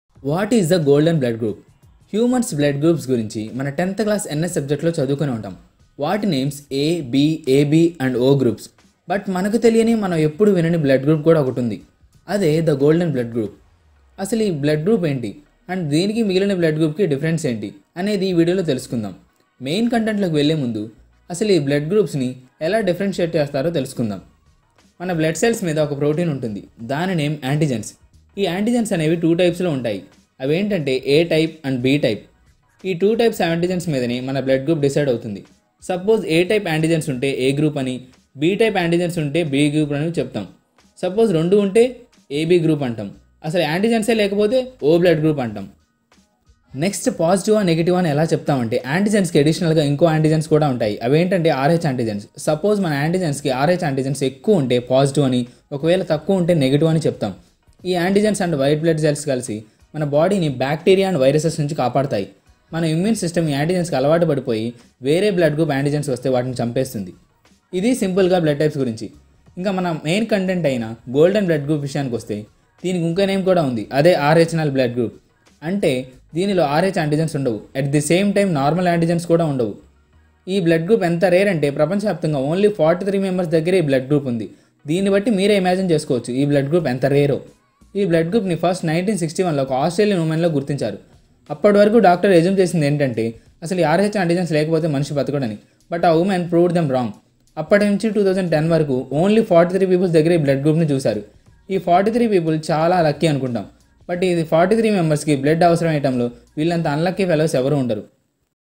Rarest Blood Group In The Earth || One Minute Interesting Fact In Telugu || Episode #42 ||FactsMaava
In this video i am going to tell you about the rarest blood group in the world.
Note: Images and videos and music used in this video are belongs to respected owners not mine.